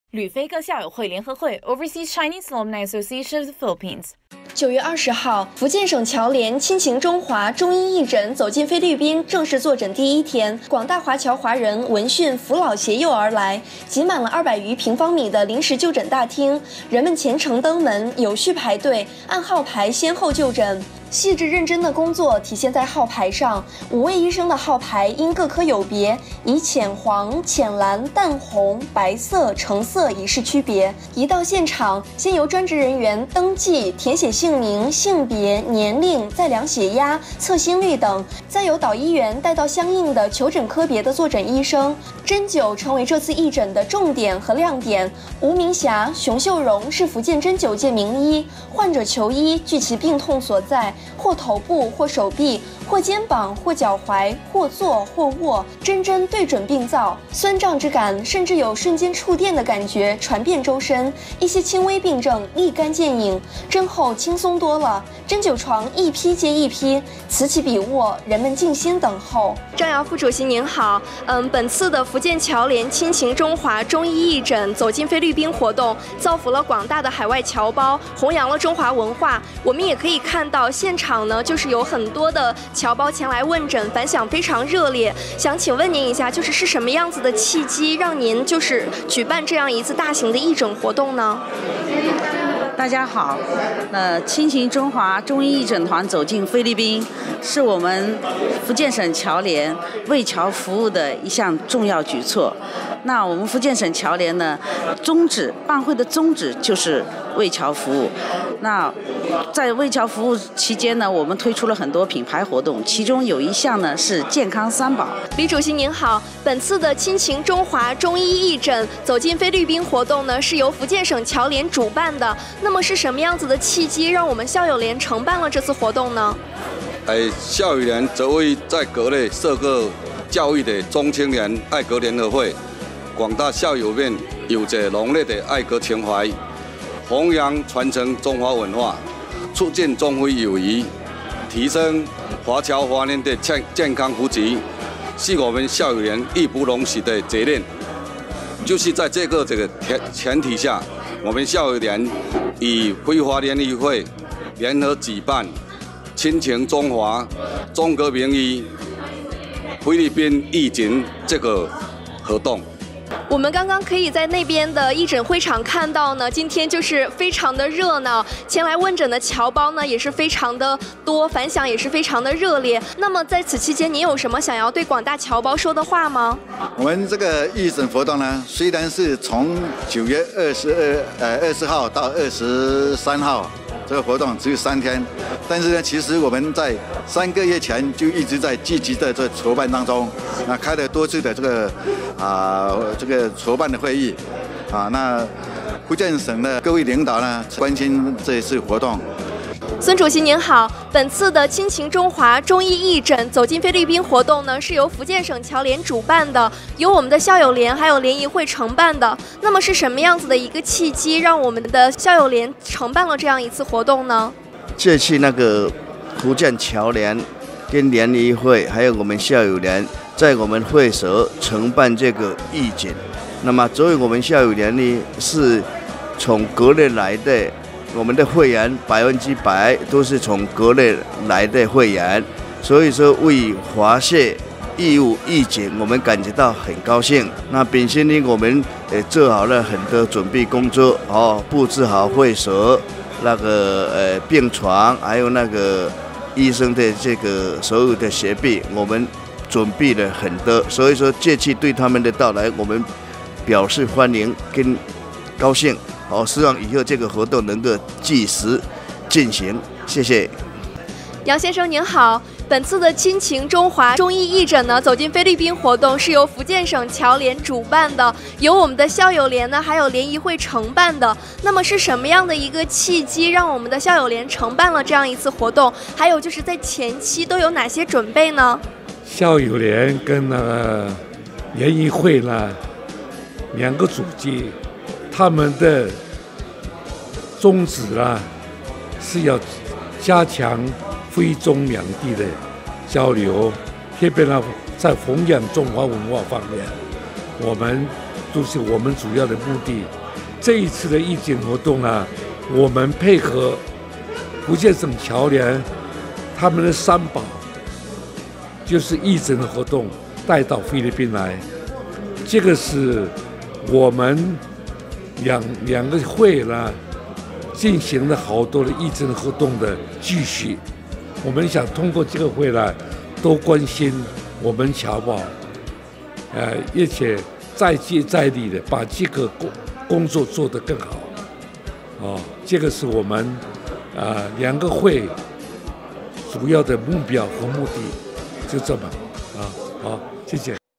Fujian Provincial Overseas Chinese Federations
Chinese Medicine Clinic

Watch this feature on Chinatown TV 菲中电视台 on the following timeslots:
Sunday 10:00-11:00 am
Wednesday 7:00-8:00 pm
IBC 13